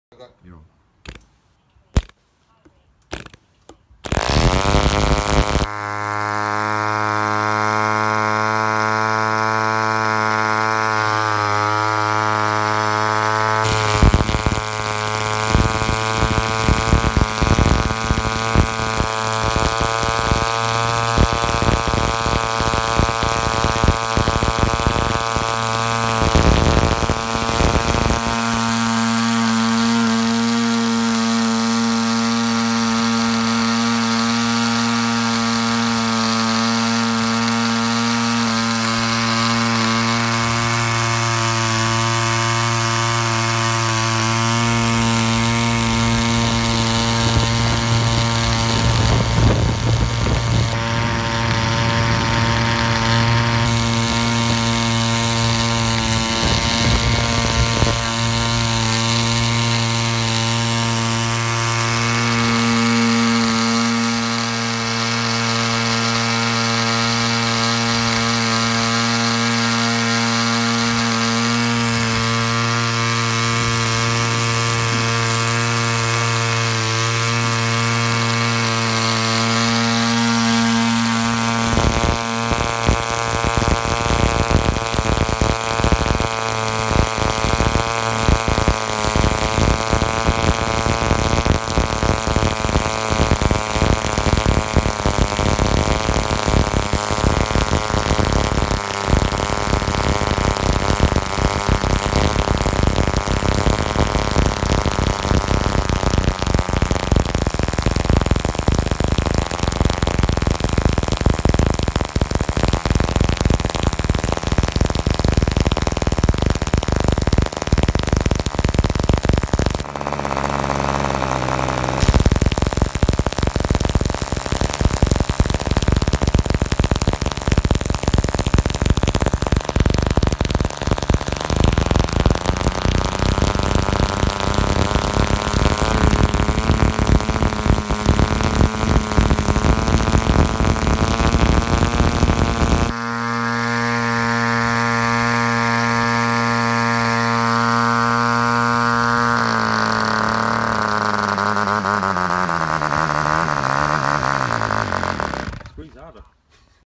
BollAero 18 1.8 c.c. piston ported model diesel engine
The "BollAero 18" is a compression ignition (aka model diesel) 1.8 c.c. model engine designed by Chris Boll for home construction from bar stock materials.

This example has ben completed by a local enthusiast to very high standard. His prior experience is in making live steam engines.

It is true to the published plan. The result is excellent in all respects. It was the first build of a model diesel engine for the constructor. The only non-home made items in the engine are the machine screws fastening the front housing, rear cover and cylinder jacket to the block.

It starts easily, runs smoothly and generally performs very much like a Mills 1.3 model diesel engine.

Fuel Composition:

25% Castor Oil

35% Di-ethyl Ether (solvent grade)

38.5% Kerosene

1.5% 2-Ethylhexyl Nitrate (Amsoil Cetane Boost)